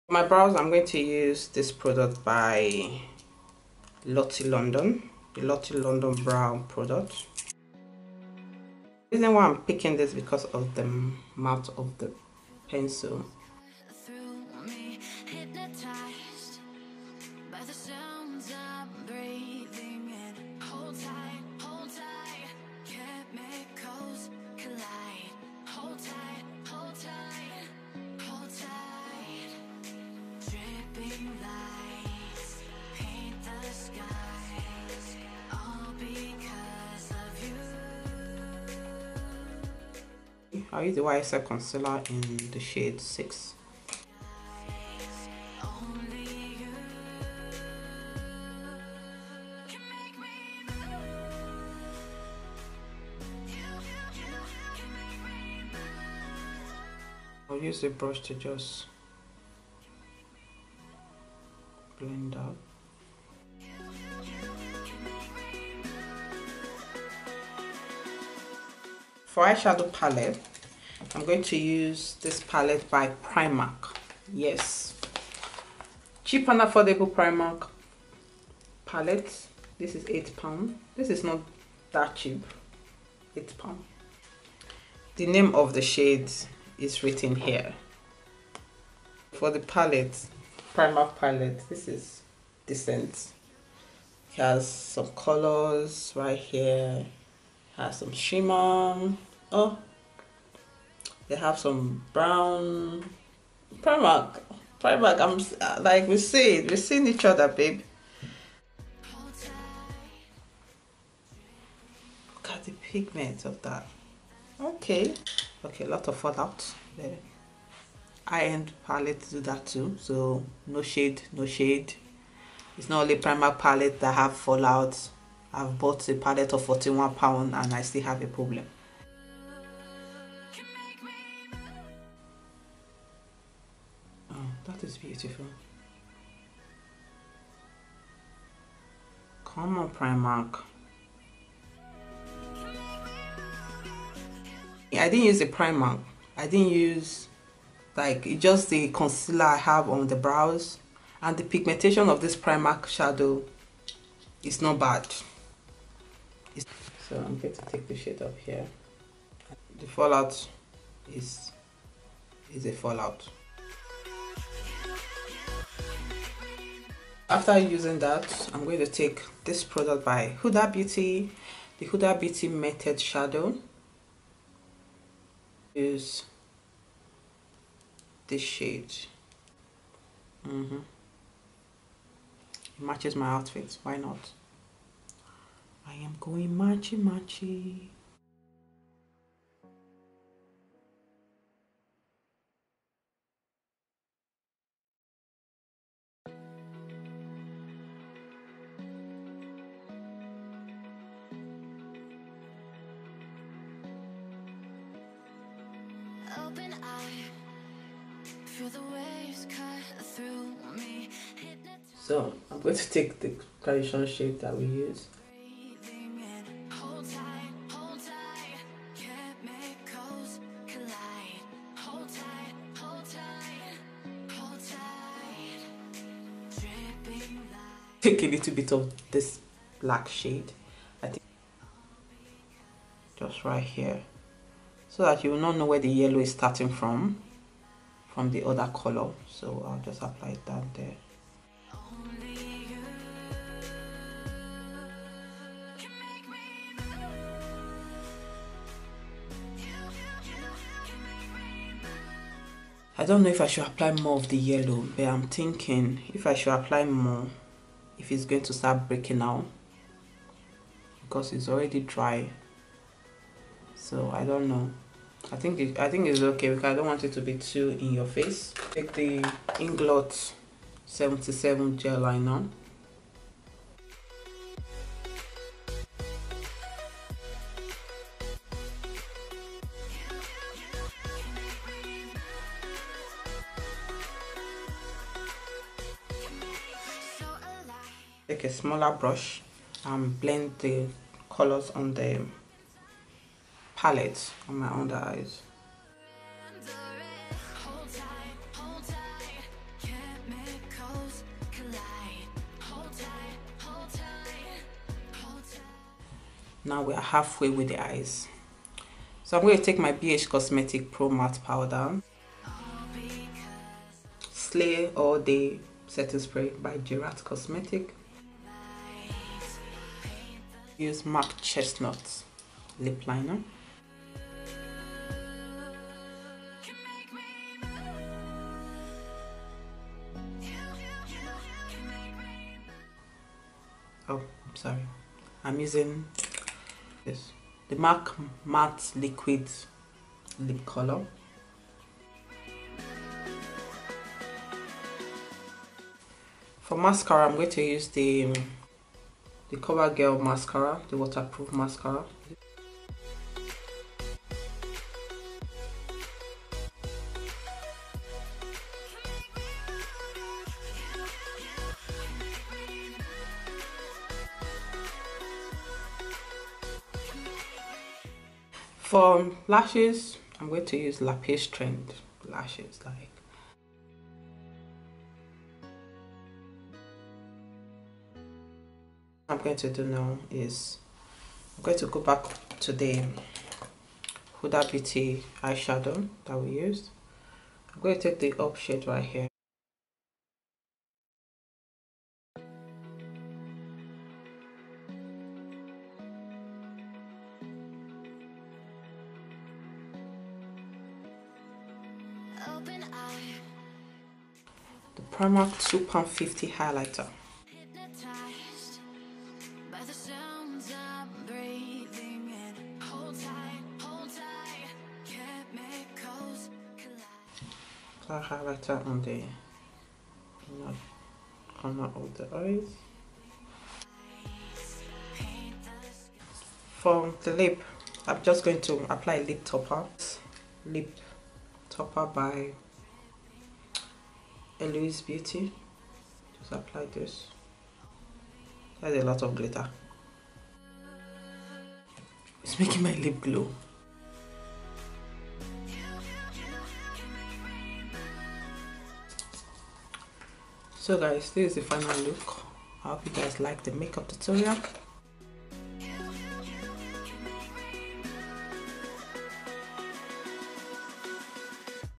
Yellow Makeup Tutorial For Dark Skin | Huda Beauty Melted Shadows & Primark Makeup
BIBLE VERSE...
I Am A Child Of Destiny Romans 8:29-30

Products USED On This Video.
Lottie London Arch Rival Microblade Brow - Warm Brown & Ebony
Primark Rose Quartz Eyeshadow Palette
HUDA BEAUTY MATTE & METAL MELTED SHADOWS - Limelight & Gold Chains
Primark PS Gold Digger Highlighter
Mac Chestnut Lip Liner
Mac Cosmetics Retro Matte Liquid Lipstick - Lady Be Good

Lighting: Amazon Ring Light
Editing Software: iMovie - iPad Pro